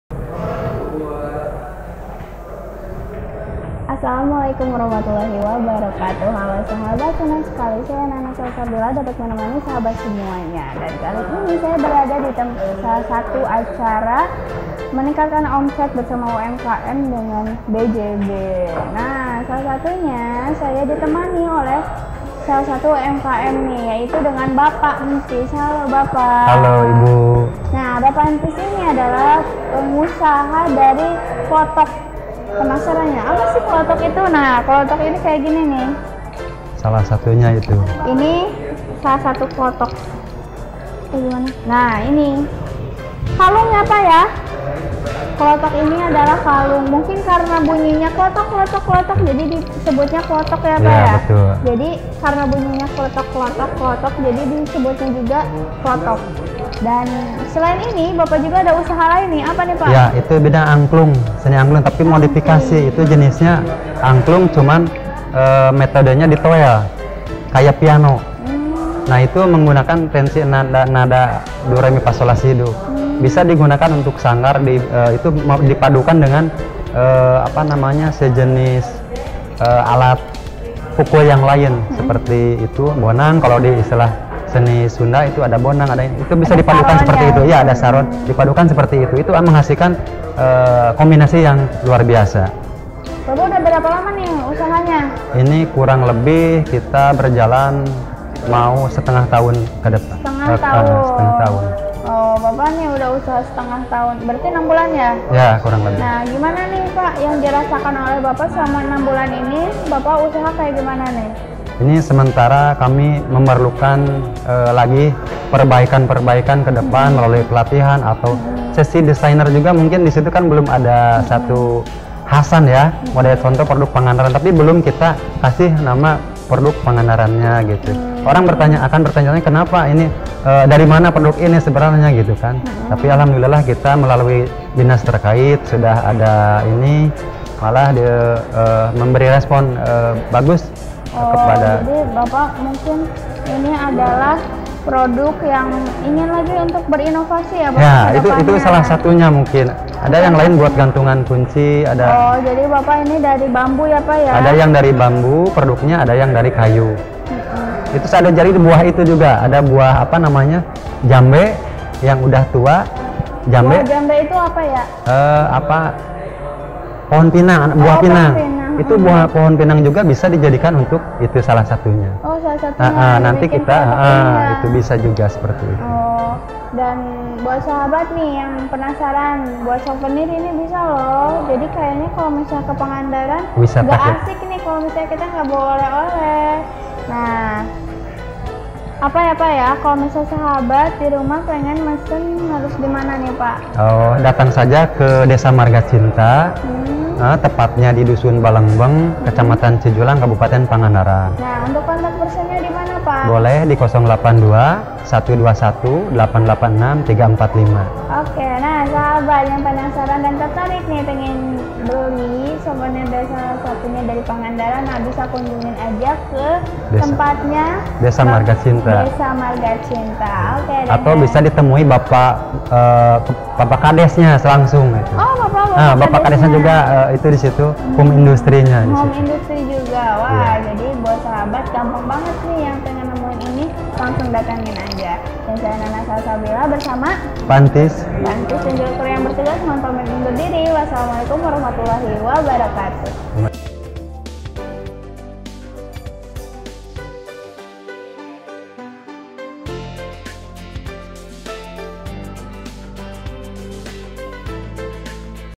WUB : Pengrajin Klotok
"Wirausaha Baru" bersama Bpk. Entis (Pengrajin Klotok)
Strabiz TV